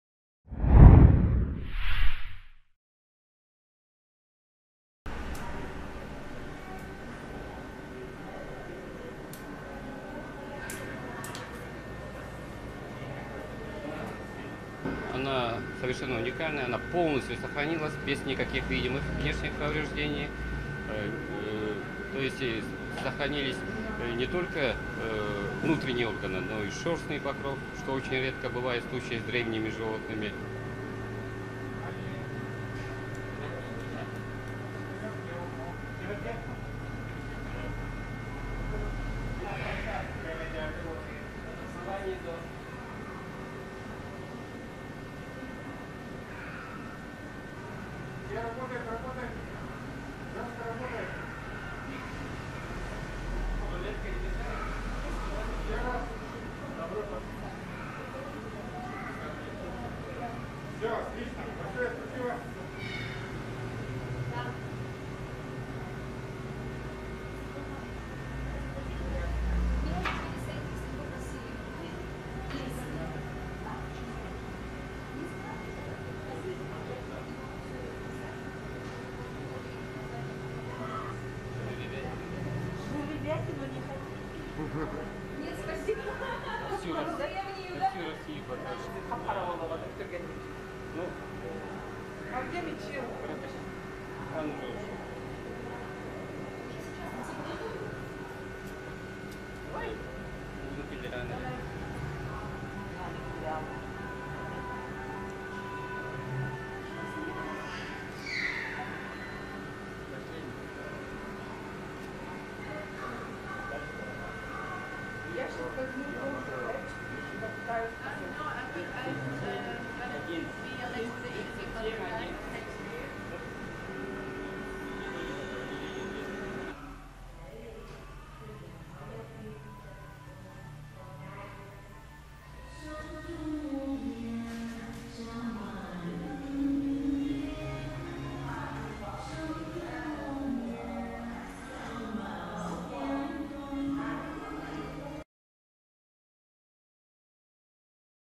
Scientists discover 40,000 year old horse in Siberian permafrost
* Channels describe the latest news in the country and the world. A collection of life, humor, unexpected situations in the world.
The 40,000-year-old baby horse dug out of the permafrost in Siberia forty millennia after the rest of its species went extinct